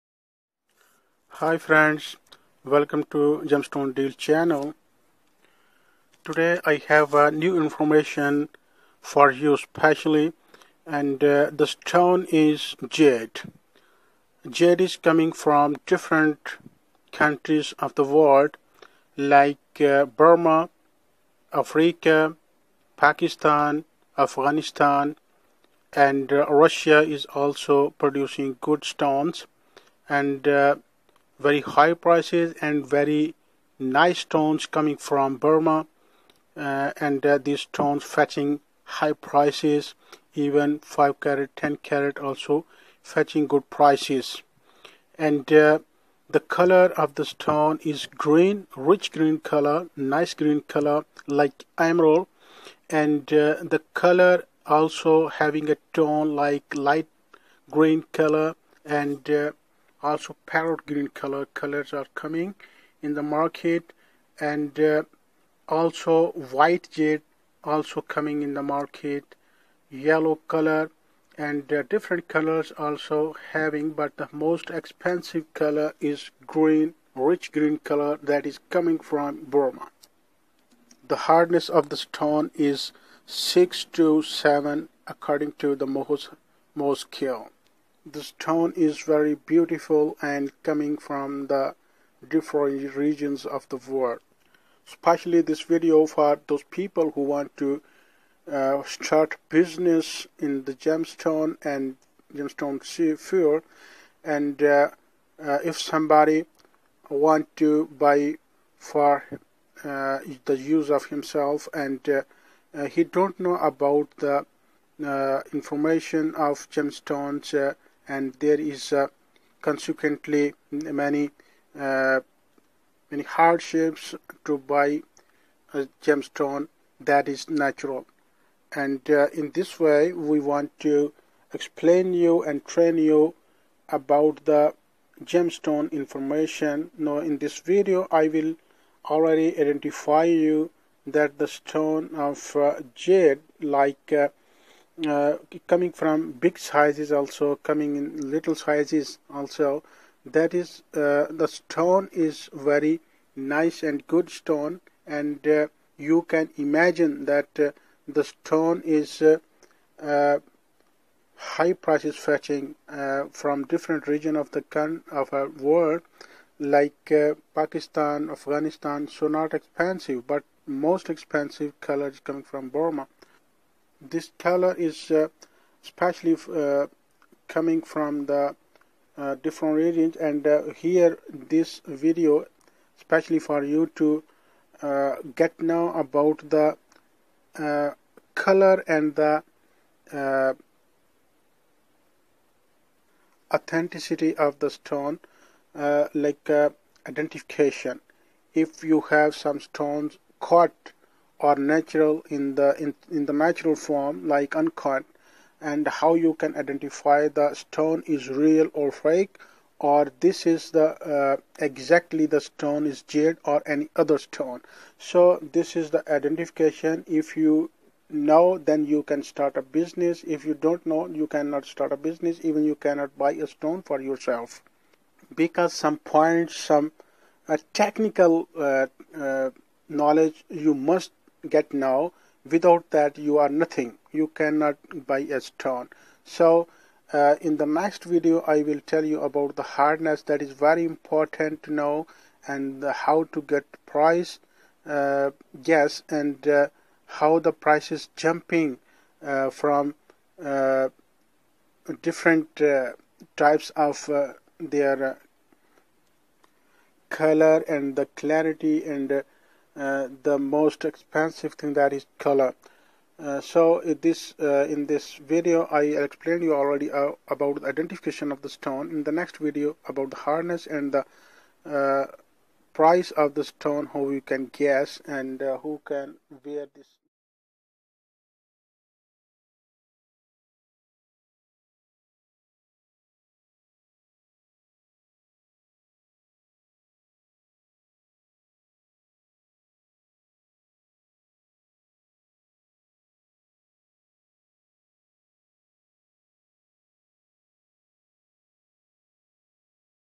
How to identify Jade Gemstone.
Jade can be distinguished from other similar materials by its hardness and density. There are a lot of other materials fraudulently sold as jade and it is difficult to identify jade by outside appearance. The most reliable method of identifying jade from other substances is by testing its specific gravity. A simple test to distinguish jadeite from nephrite is a chime test. Nephrite emits a musical tone when it is struck, whereas jadeite does not.
Nephrite is more common than jadeite and deposits have been found in New Zealand, Australia, Brazil, China, Canada, Zimbabwe, Russia, Taiwan, Alaska and Poland. The main source of jadeite is Myanmar (Burma), which is also the only source of imperial jadeite. Jadeite is also found in Japan, Canada, Guatemala, Kazakhstan, Russia, Turkey, Cuba and the USA.
And more we will Disscuss in this video.